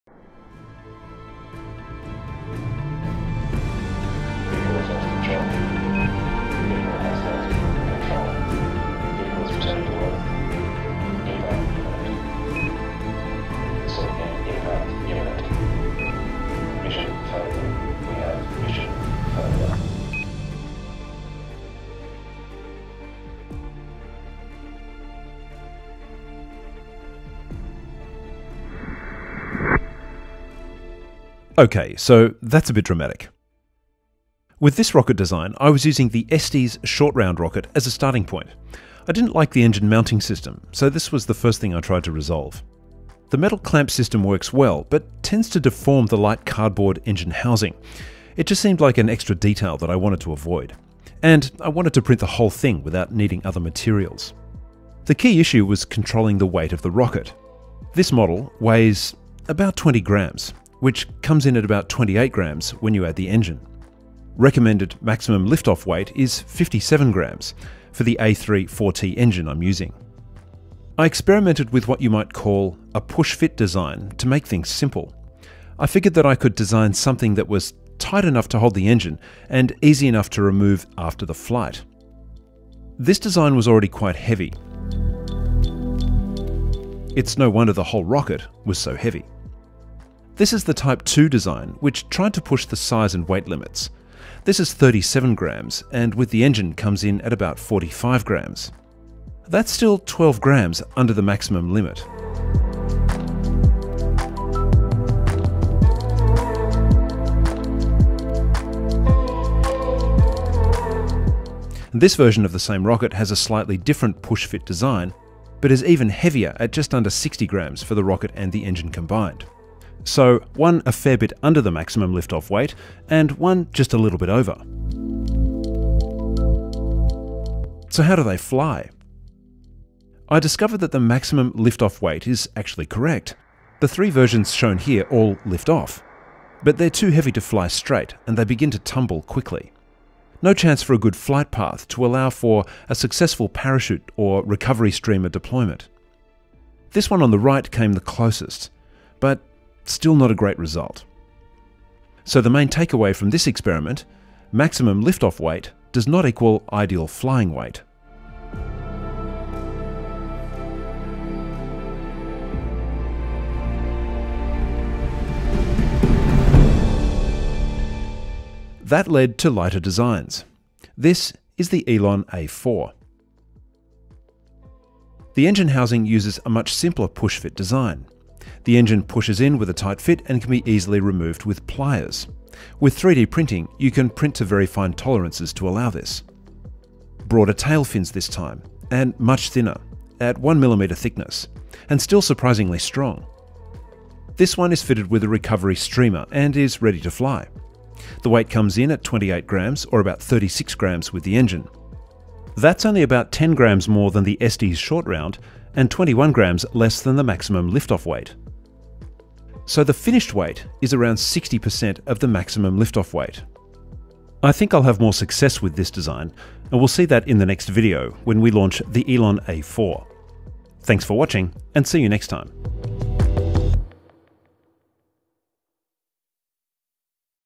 ROCKET FAIL | 3D printed rocket test and evaluation | Design & Technology
This rocket design explores the process of testing assumptions about how weight affects flight. The Rocket uses the Estes A3-4T engine. The main lesson learned is that maximum lift-off weight does not equal ideal flying weight.